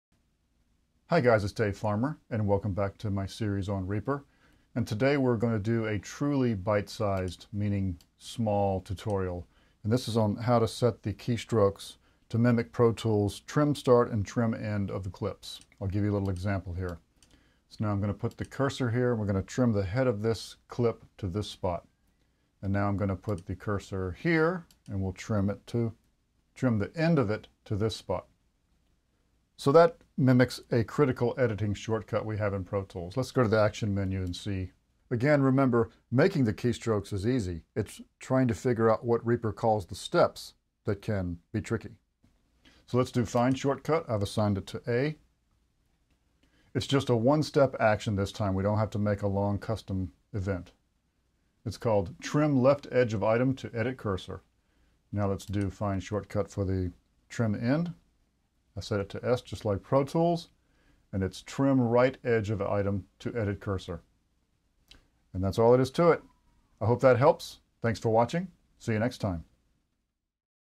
Reaper 011 - Trim Start and End of clips
A quick tutorial showing the action step required to mimic the Pro Tools shortcut "Trim Clip Start & Trim Clip End".